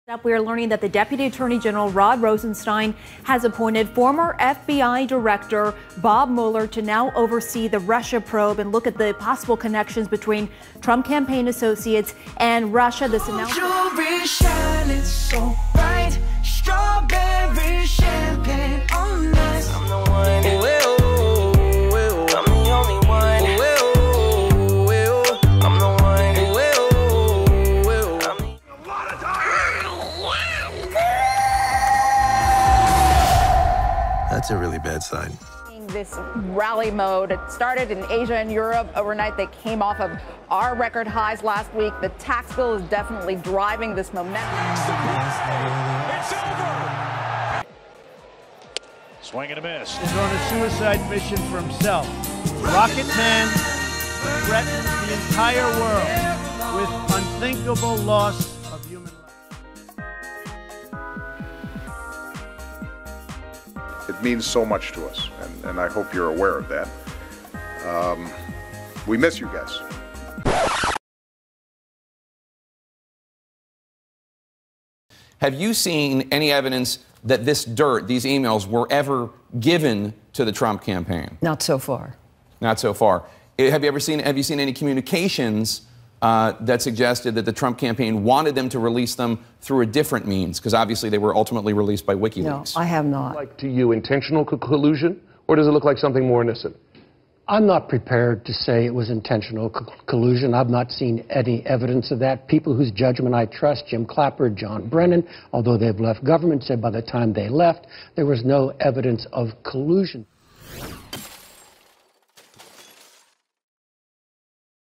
The Mueller Investigation: One Year Later
The Mueller investigation into collusion between the Trump campaign and Russia has been going for over a year now, what have we found?  One year later we haven't discovered any information that ties the Trump campaign to Russia.